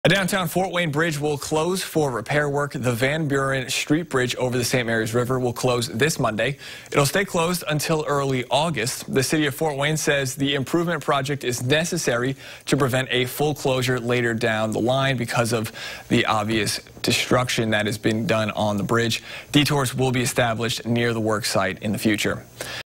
Fort Wayne's Public Works Division to begin Van Buren Street Bridge upgrades Jan. 11
The City of Fort Wayne's Public Works Division on Thursday announced enhancements to the Van Buren Street Bridge over the St. Marys River will begin next week.